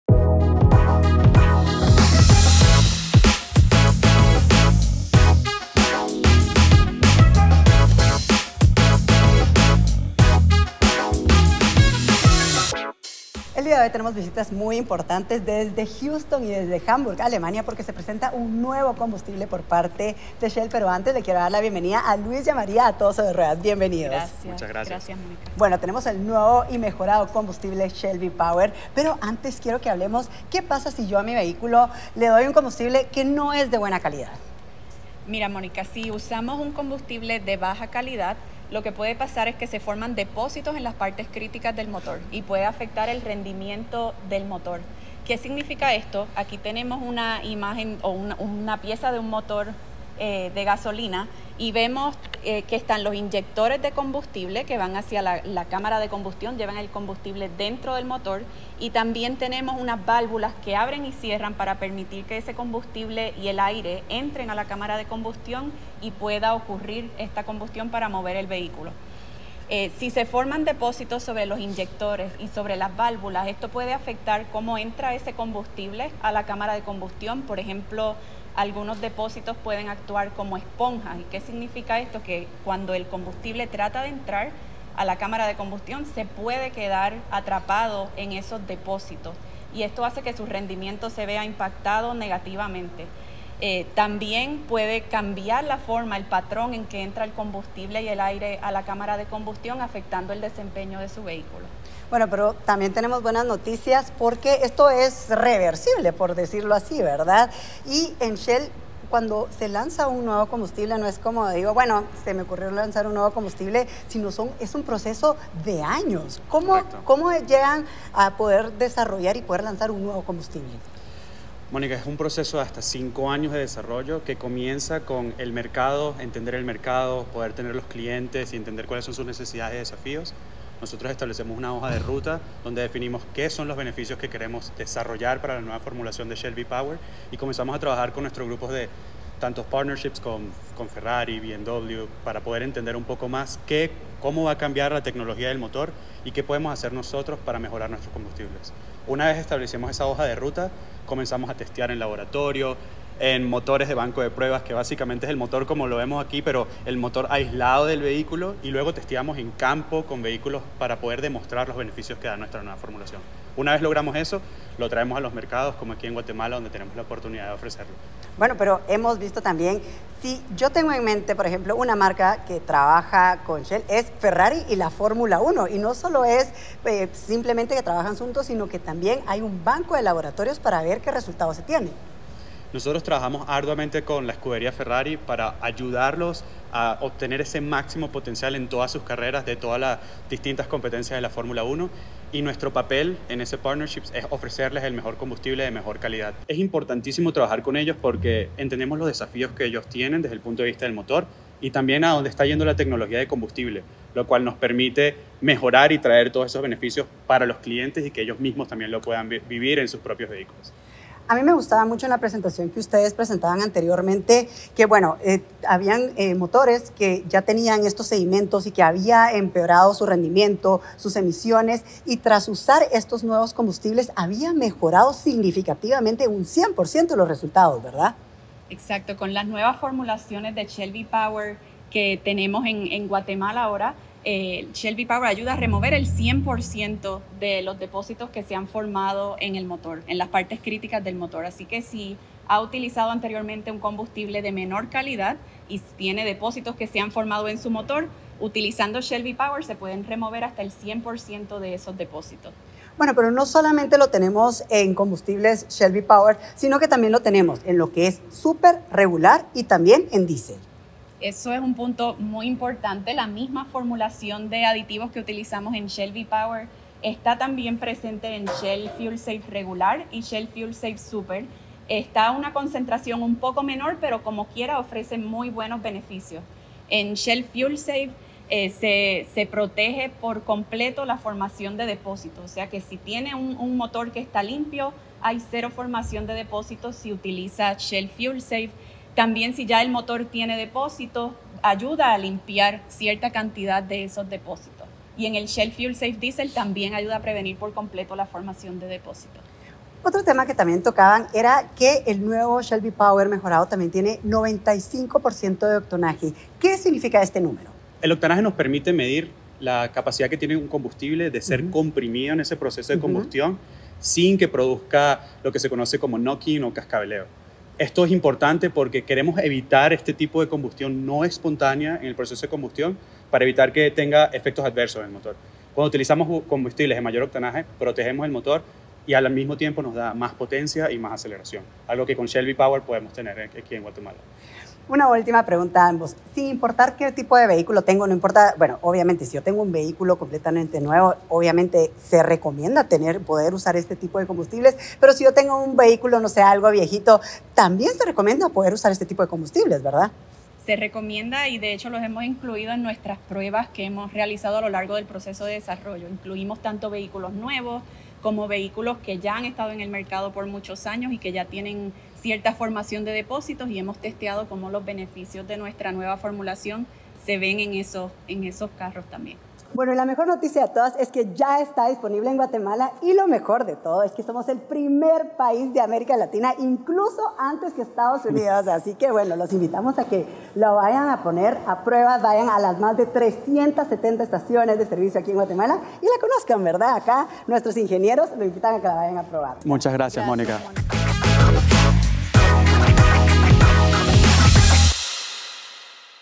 Llega a Guatemala la nueva formulación de Shell V-Power
Shell, la marca líder de combustibles a nivel mundial, informó que Guatemala es el primer país en América donde Shell lanza la nueva formulación en todos sus combustibles para compartir con los consumidores guatemaltecos los beneficios únicos que ofrece esta innovación tecnológica.

Luis Alayón, Científico de Combustibles de Shell Global Solutions en Shell Technology Center de Hamburgo y María Rodríguez-Moyá, Científica de Combustibles de Shell Global Solutions en Shell Technology Center de Houston, nos explican todos sus beneficios.